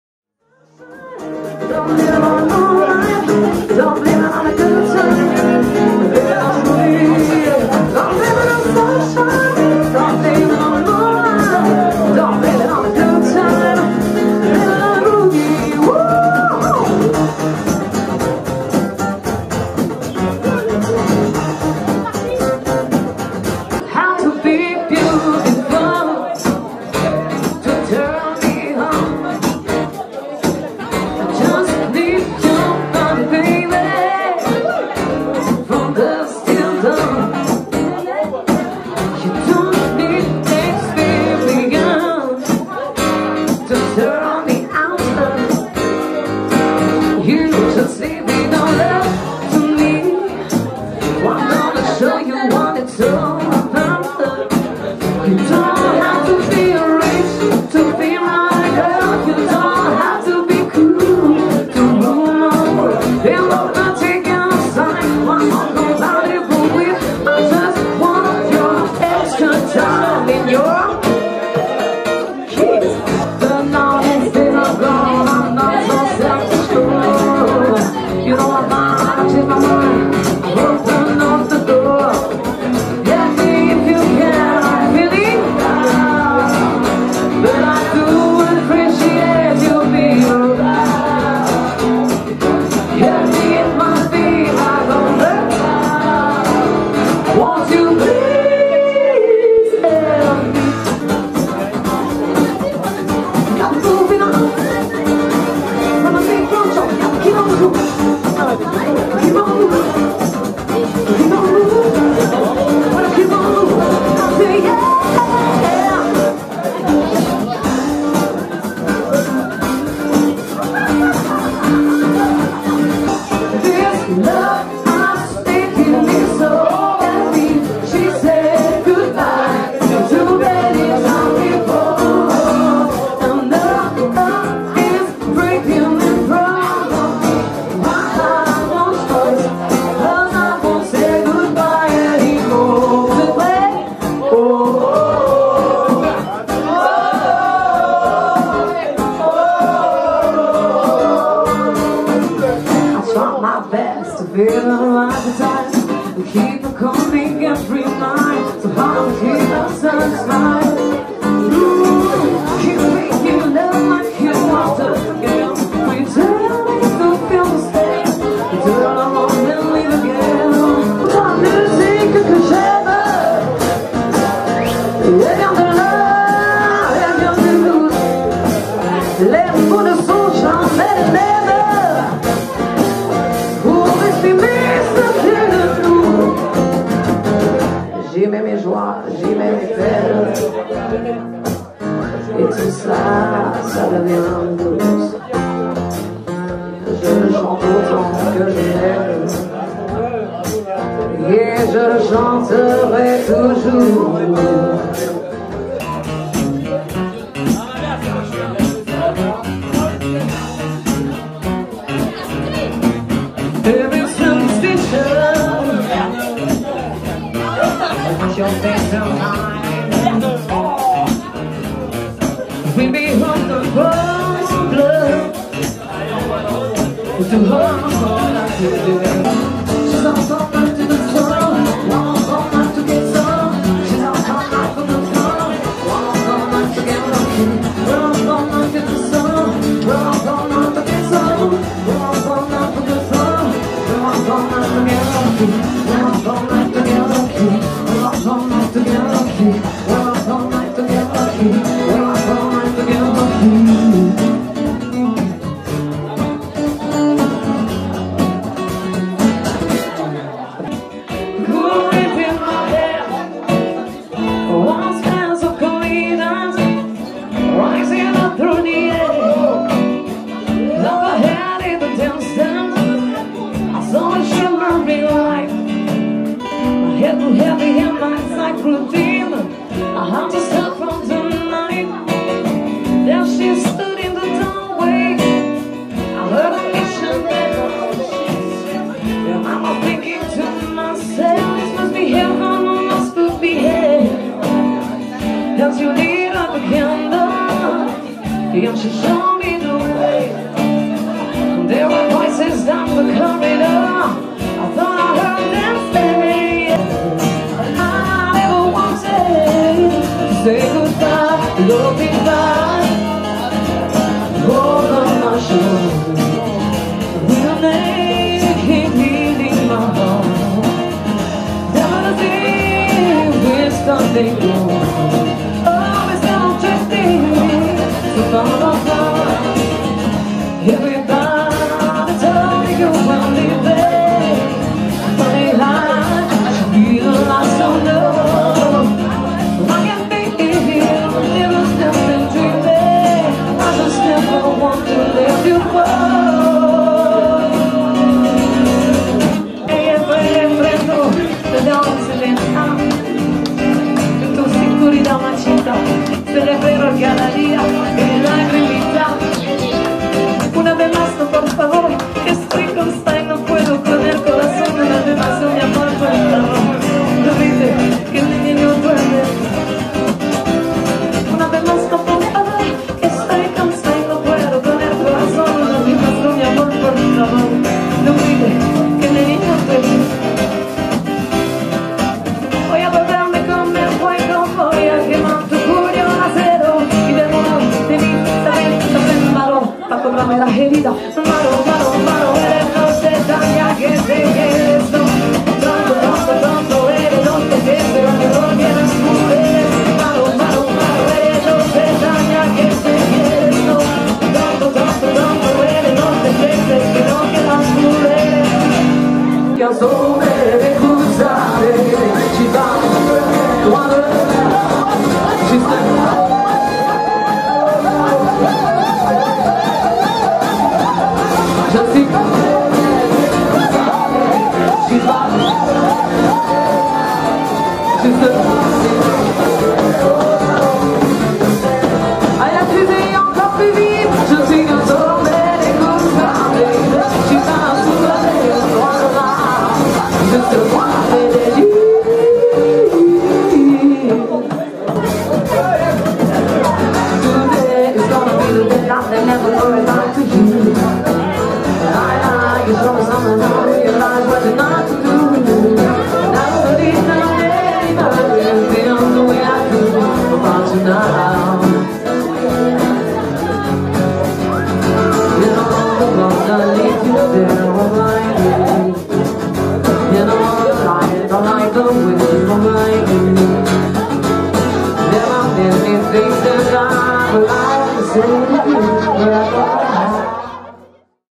DELIGHT TRIO - Live au Sinclair (Novembre 2013)
Concert DELIGHT Trio : Extraits live
Chant Lead : Maggy Trottier / Guitare : Romain Leray / Percu & Batterie : Benjamin Leray